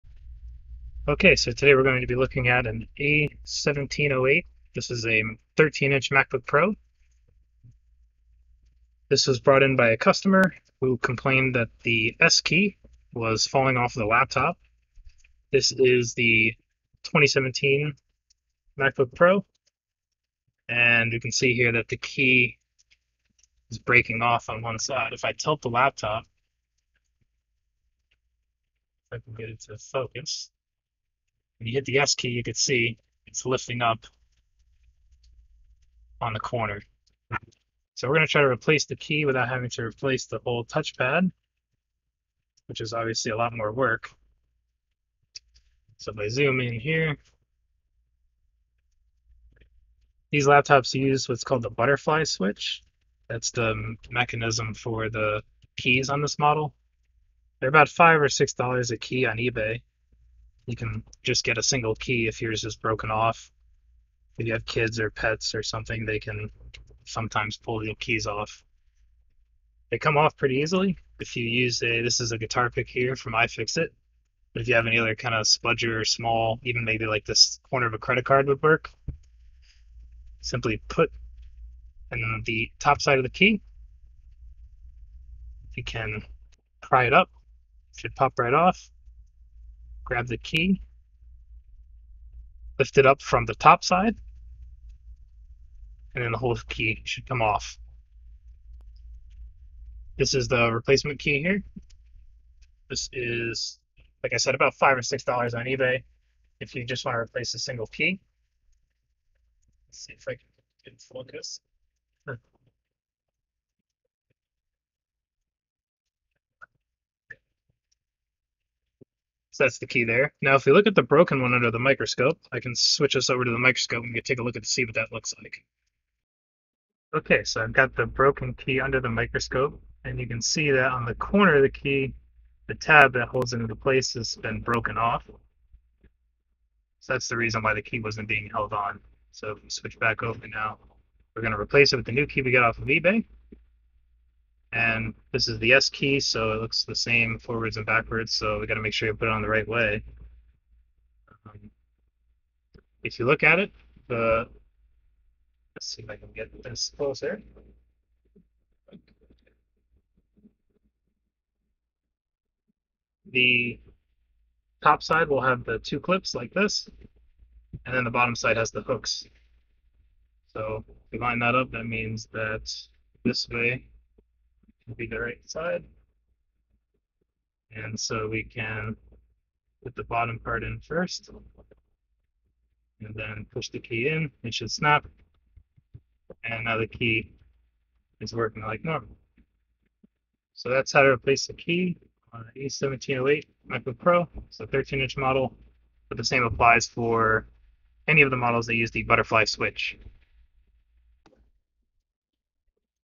How-to Replace a butterfly letter key on an Apple Macbook Pro A1706 / A1708 2016 / 2017 models
How to replace a missing/broken key on an Apple Macbook Pro A1706 / A1708 for cheap without having to replace the entire palmrest assembly.